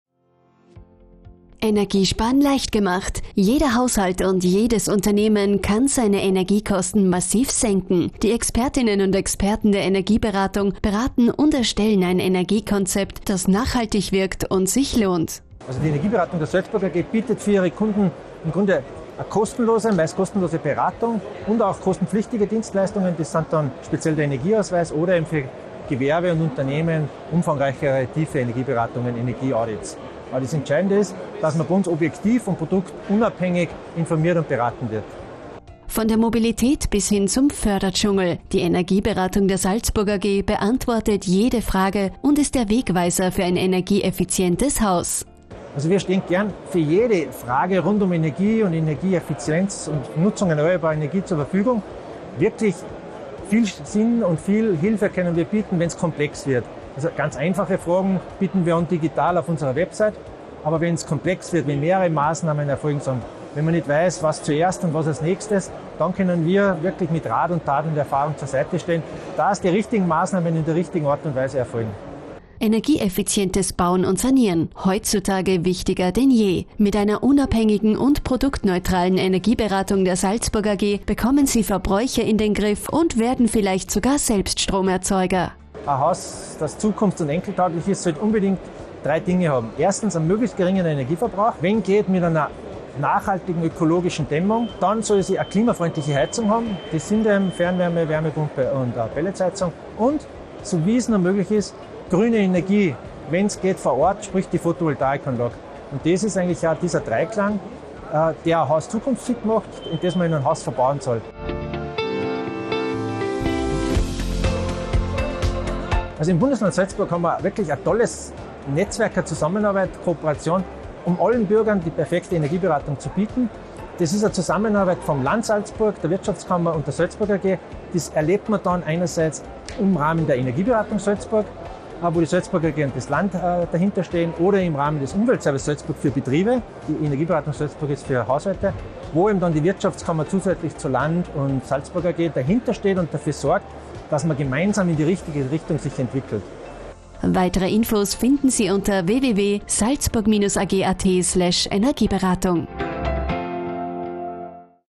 Bauen und Wohnen 🏡 - Energieberatung⚡
ENERGIESPAREN LEICHT GEMACHT - Jeder Haushalt kann seine Energiekosten um circa 30% senken. Vom Energieausweis, über Fragen zur Heizung bis hin zur eigenen Photovoltaikanlage -  Die Expertinnen und Experten des GreenTech Unternehmen Salzburg AG beraten Sie gerne und erstellen ein Energiekonzept, das nachhaltig wirkt und sich lohnt.

00:00 Einleitung
00:15 Energieberatung
00:35 Fragen
01:12 nachhaltig bauen & sanieren
01:51 Energieberatungsnetzwerk
02:27 Abschluss